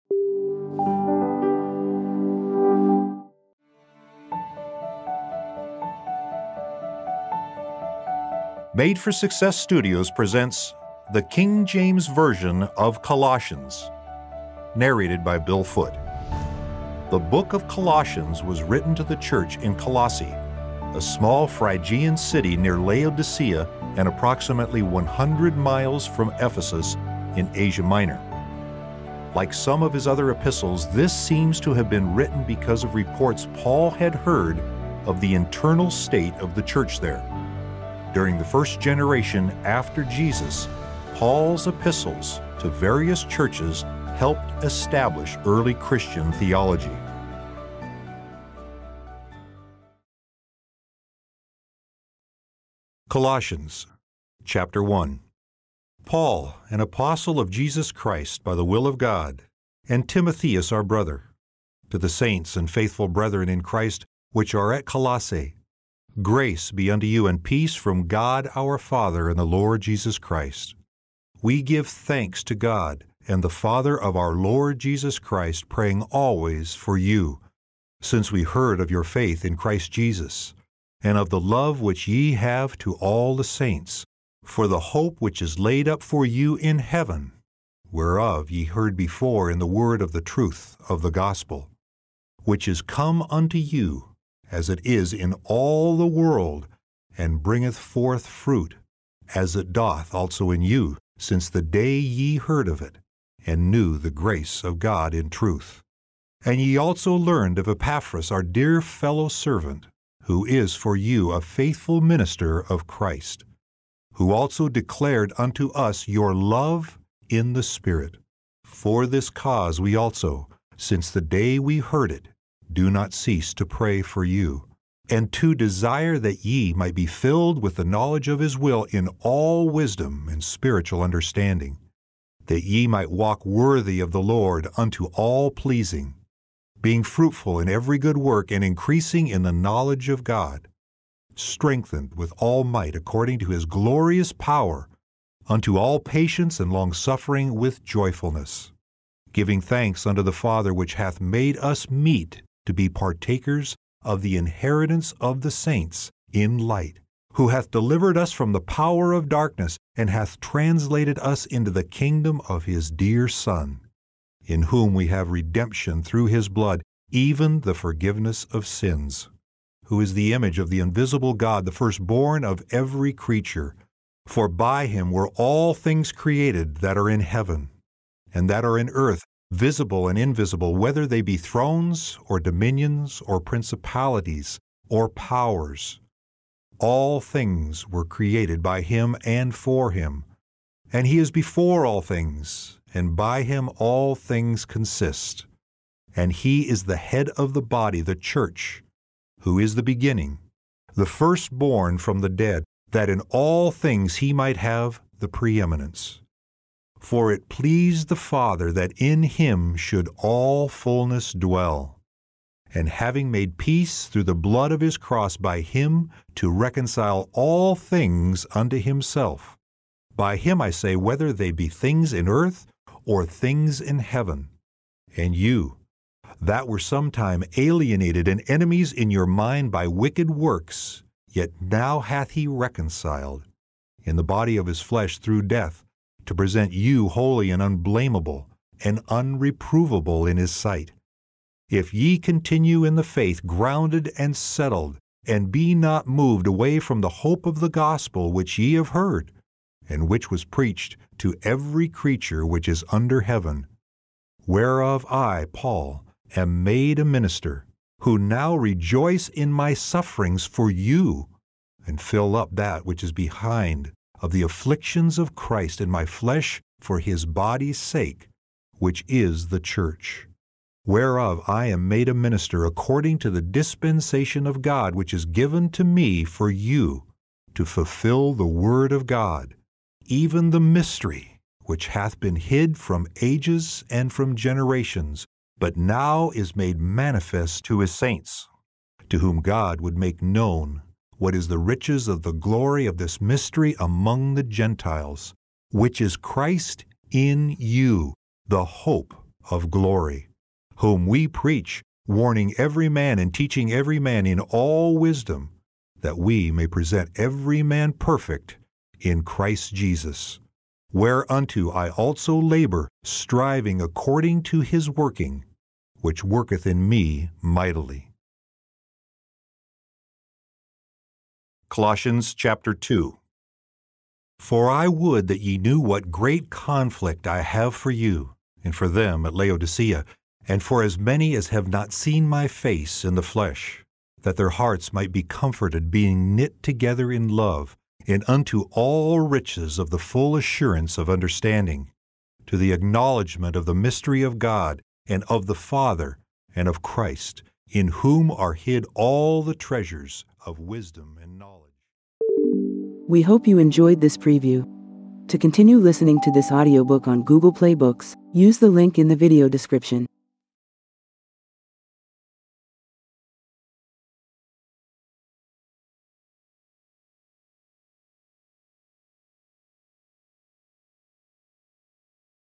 Book of Colossians: King James Version Audio… by Unknown author · Audiobook preview
Book of Colossians: King James Version Audio Bible
Narrated by Bill Foote

0:00 Intro
0:03
0:52
5:21
5:52 Outro

BOOK DESCRIPTION

The Book of Colossians was written to the Church in Colossae, a small Phrygian city near Laodicea and approximately 100 miles from Ephesus in Asia Minor. Like some of his other epistles, this seems to have been written because of reports Paul had heard of the internal state of the church there. During the first generation after Jesus, Paul's epistles to various churches helped establish early Christian theology. English speakers are acclaiming American voice artist Bill Foote for his caring read of God's word, making the complete King James audio Bible easy for today's listeners to understand the Word of God.

Bill Foote is a graduate from Western Washington University with a BA in broadcasting. After a successful thirty-year career in the business world, he returned to Liberty University and graduated with a master’s degree. When he is not behind the microphone, he is announcing sports for a local high school. He resides in Seattle, Washington.

AUDIOBOOK DETAILS

Language: English
Publisher: Made for Success
Published on: July 14, 2020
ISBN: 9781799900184
Duration: 0 hr, 16 min
Genres: Bibles / King James Version / General